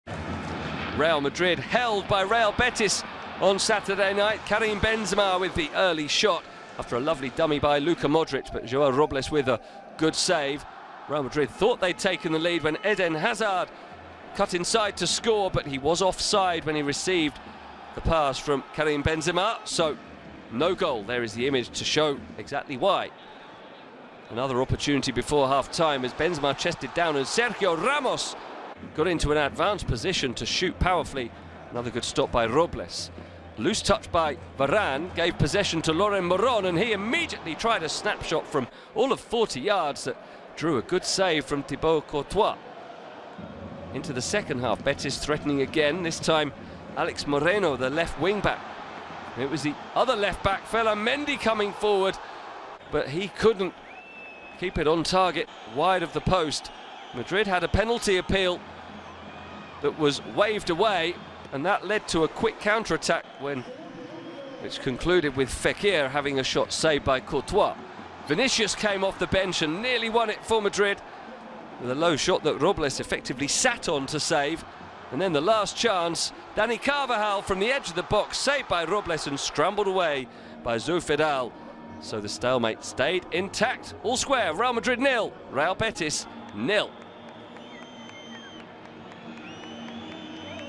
Highlights Real Madrid vs Real Betis (0-0)
Goalless draw at the Santiago Bernabeu stadium between Real Madrid and Real Betis that leaves the local team without the lead RealMadridRealBetis J12 LaLiga Santander 2019/2020